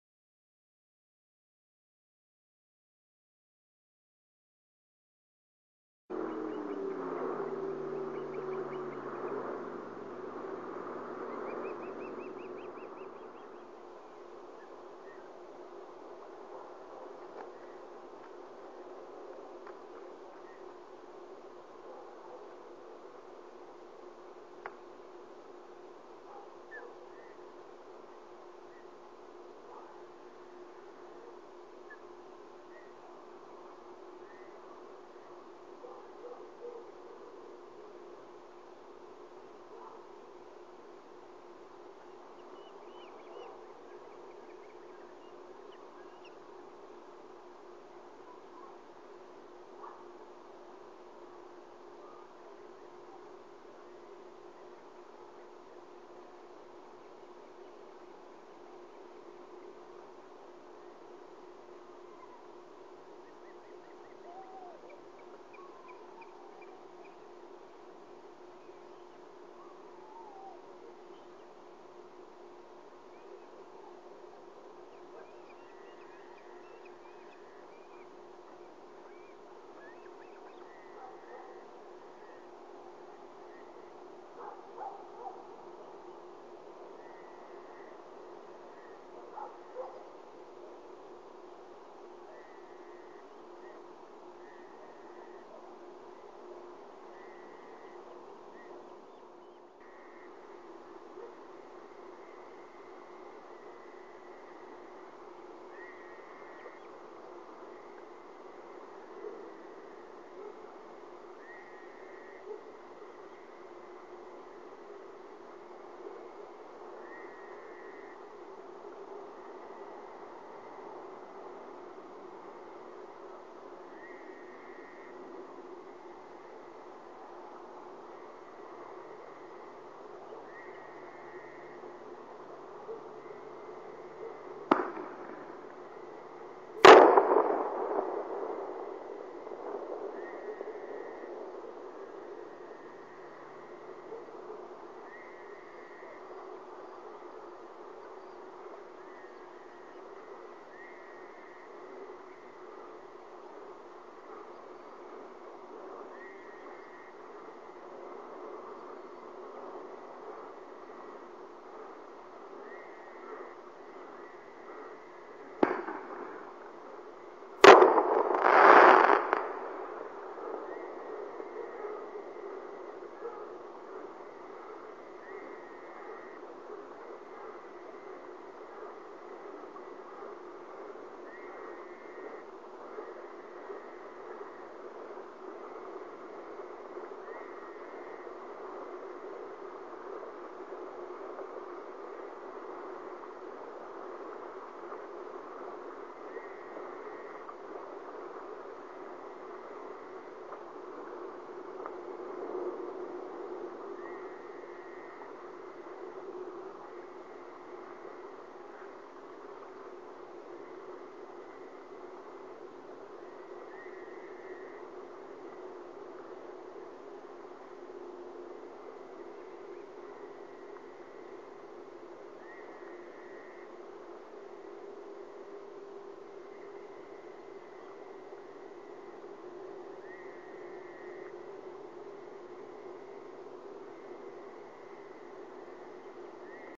Lake Murray Osprey Cam Lucy brings in fish 3 Twinkle helps herself 8:54pm 6-2-2019
Lucy was on the nest and she left at 8:29pm. After 25 mins she returned with a fish that was very much alive (so she caught it). Twinkle reminded Twilight that she gets to eat first. But no to worry Twilight did the trick of going to the opposite side of Lucy and was getting some good bites. Twinkle was helping herself to bites while Lucy was feeding Twilight. Twinkle did get a few but it is not as easy as Lucy makes it look. Someone was shooting off fireworks and according to LMO "Ricky and Lucy have dealt with fireworks for at least 7 years. I don't let anyone fire any on our property. So they are safe." -- and Lucy was not bothered about them at all